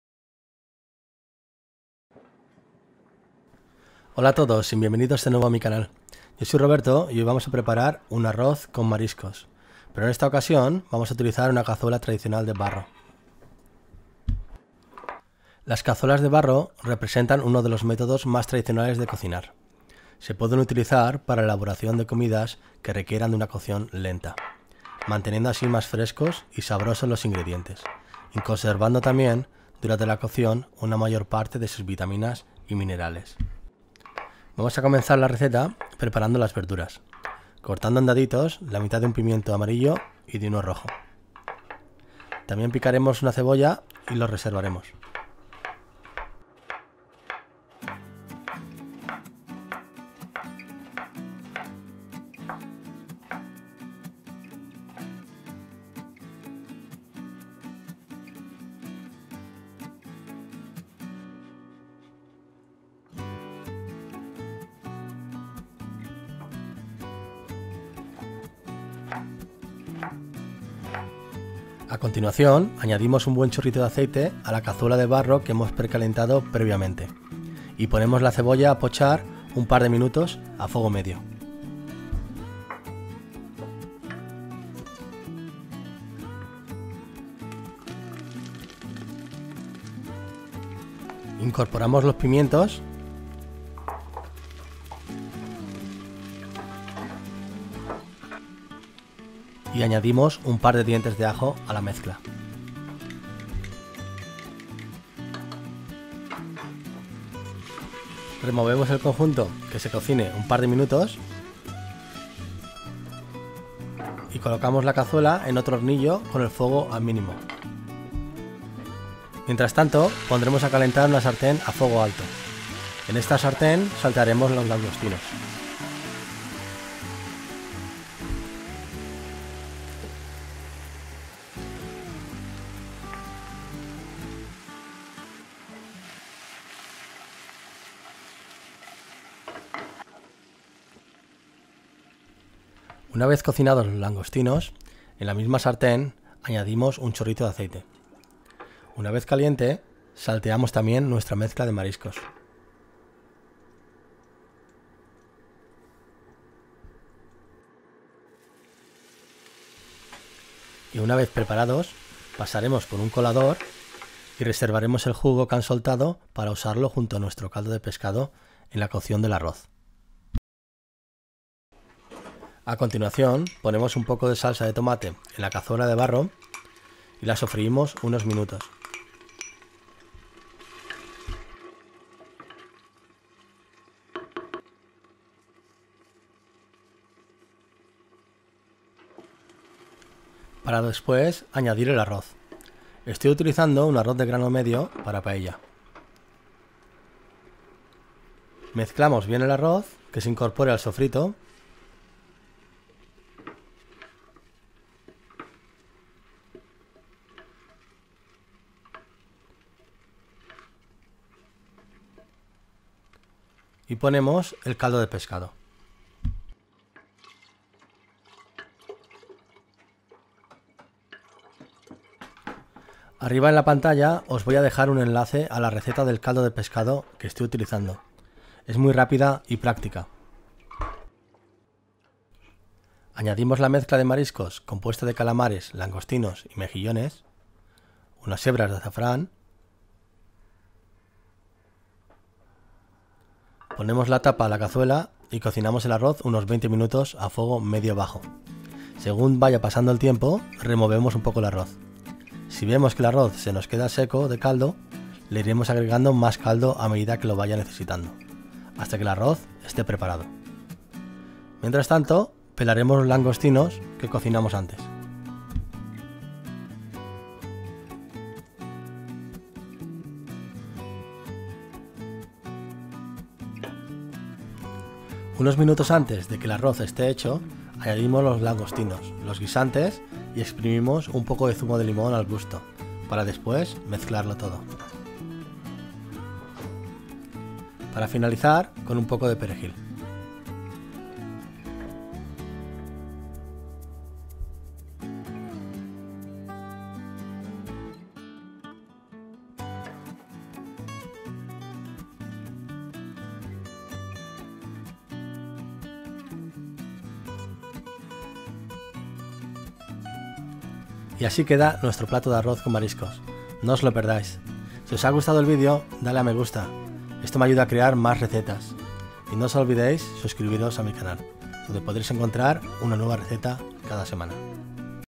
Como hacer un ARROZ de MARISCOS [facil y rápido] | En Cazuela de Barro ✅
Hoy vamos a preparar un Arroz con Mariscos, pero en esta ocasión vamos a utilizar una cazuela tradicional de Barro.
Las cazuelas de barro representan uno de los métodos más tradicionales de cocinar. Se pueden utilizar para la elaboración de comidas que requieran de una cocción lenta, manteniendo así más frescos y sabrosos los ingredientes y conservando tambien durante la cocción una mayor parte de sus vitaminas y minerales.

Ingredientes (4 personas):
Arroz para paella                                 350g
Langostinos                                         300g
Mezcla de Mariscos
(Calamar, Langostino, Mejillones)     300g
Cebolla                                                   1/2
Dientes de ajo                                       2
Pimiento amarillo                                 1/2
Pimiento rojo                                         1/2
Salsa de Tomate                                   50ml
Caldo de Pescado                                1Litre
Guisantes verdes                                 50g
Aceite de oliva virgen extra                20ml
Zumo de limón                                    Al gusto
Azafran                                                 Al gusto
sal / pimienta / perejil

LOS INGREDIENTES DE LA RECETA Y SU ELABORACIÓN PODRÁS ENCONTRARLOS EN EL BLOG